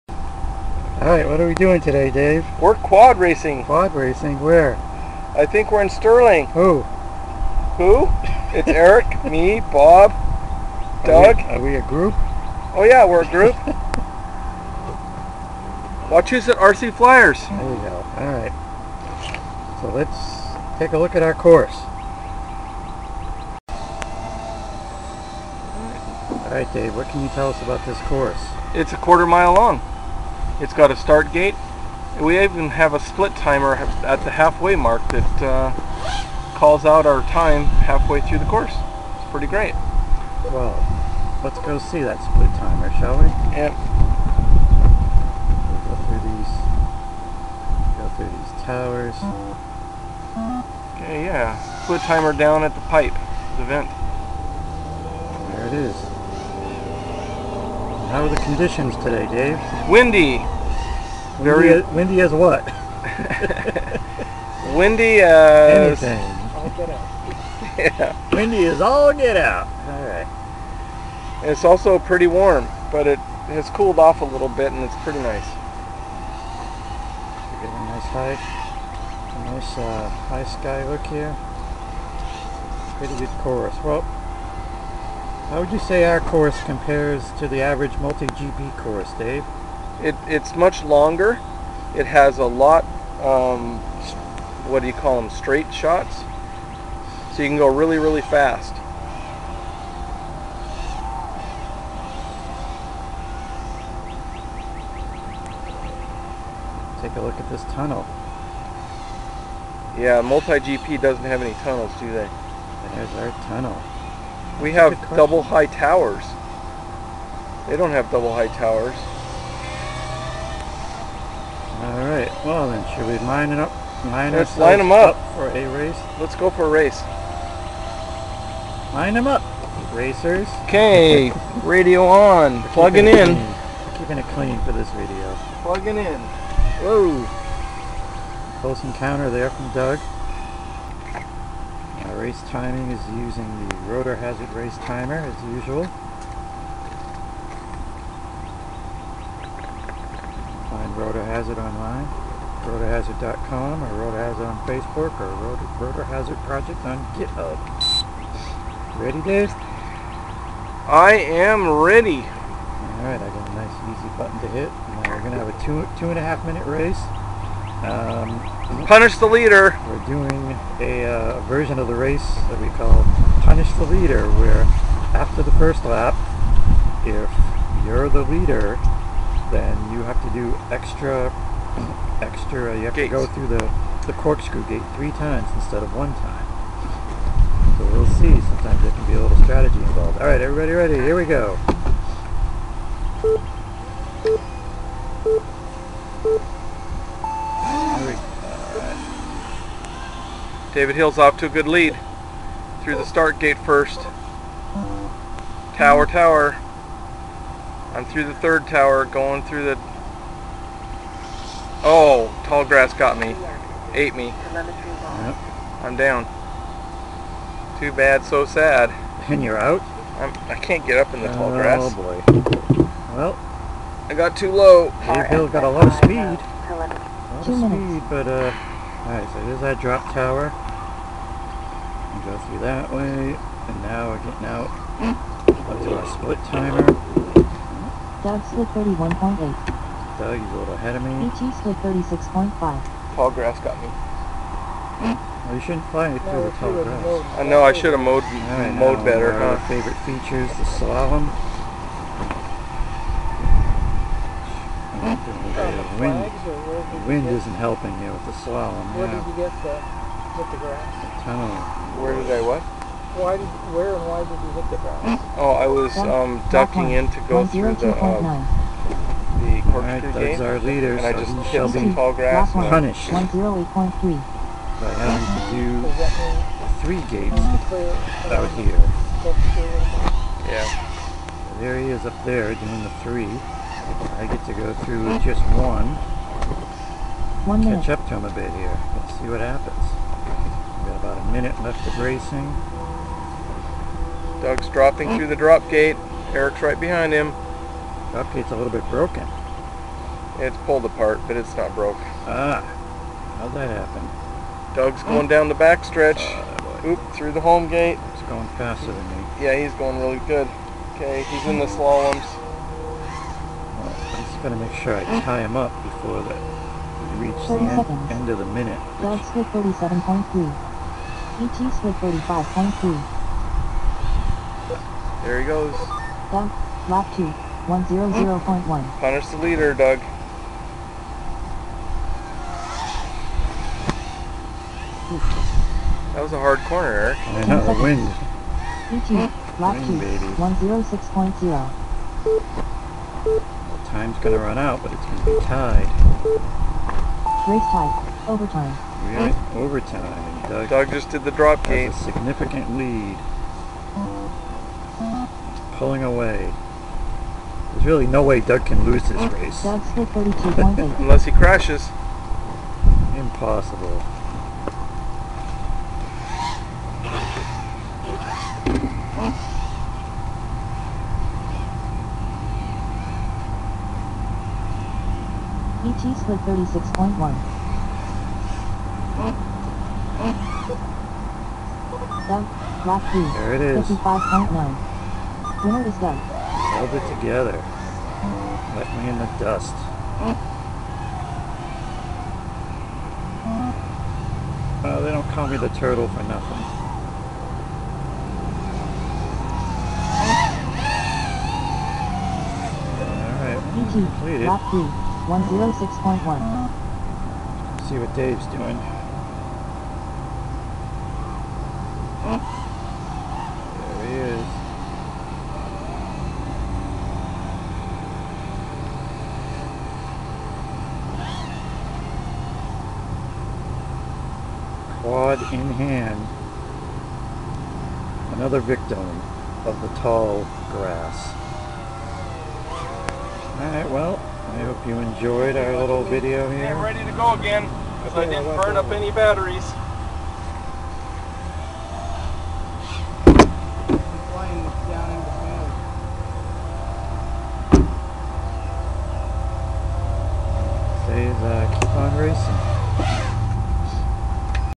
Sterling Field 2021-06-05 1
Talking about the course and racing at WRCF Sterling field.